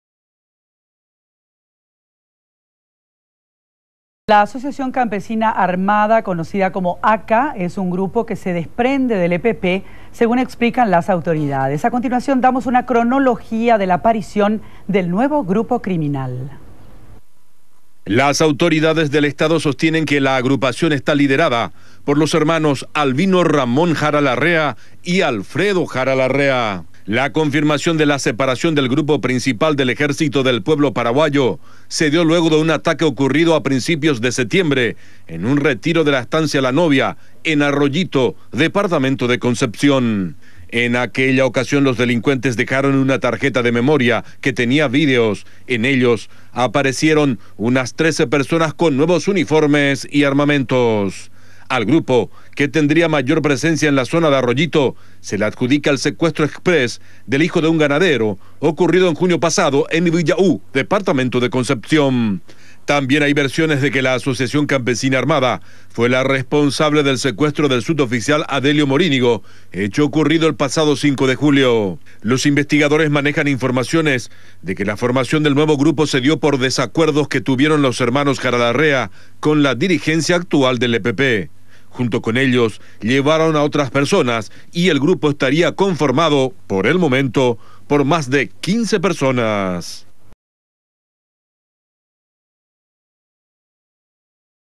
El grupo ACA sería un desprendimiento del EPP 19/09/2014
La Agrupación Armada Campesina (ACA), sería en desprendimiento del Ejercito del Pueblo Paraguayo (EPP), según las autoridades. La agrupación está liderada por los hermanos Albino y Alfredo Jara Larrea, la separación se habría dado luego de un ataque a la estancia "La Novia", en Arroyito Concepción, a principios de setiembre de este año.